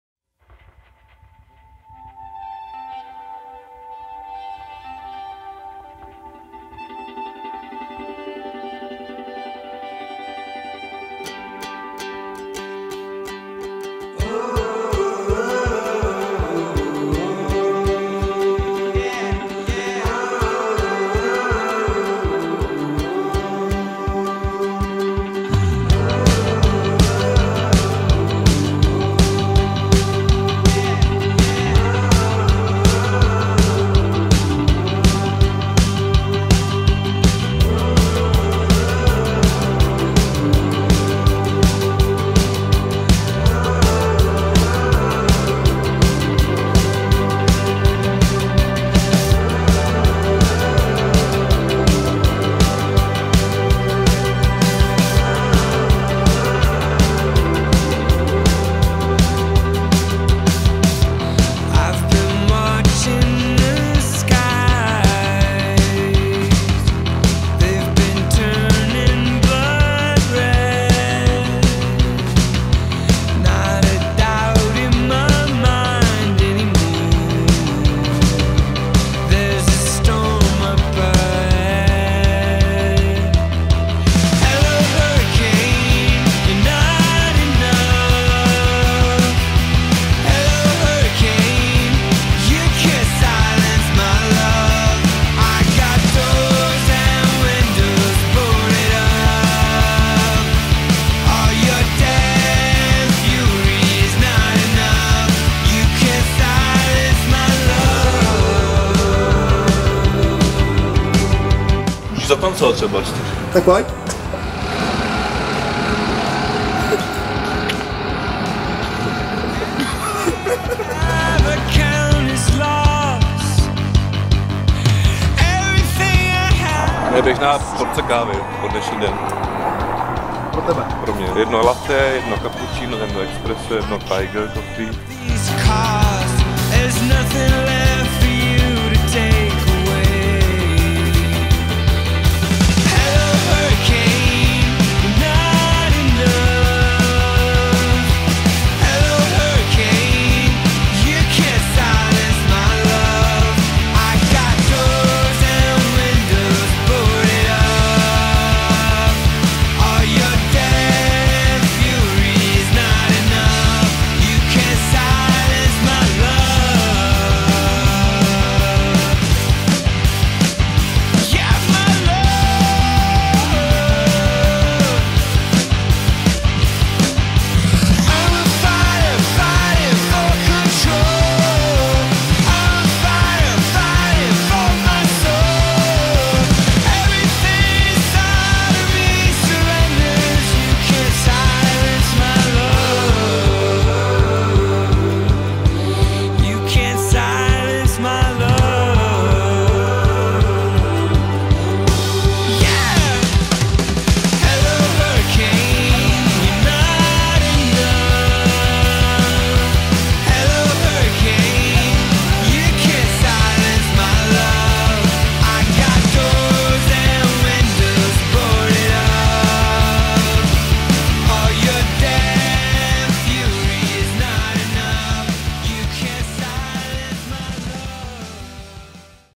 Klenoty Aurum - focení reklamní kampaně 2011
Focení probíhalo 2. a 3. dubna v pražeské nemocnici U Apolináře.

Fotograf: Lukáš Dvořák
Vlasy a make-up: Kateřina Kosová
Šaty a čepec: Ivana Mentlová
Modelka: Zuzana Stráská
& Saint-Tropez Sisters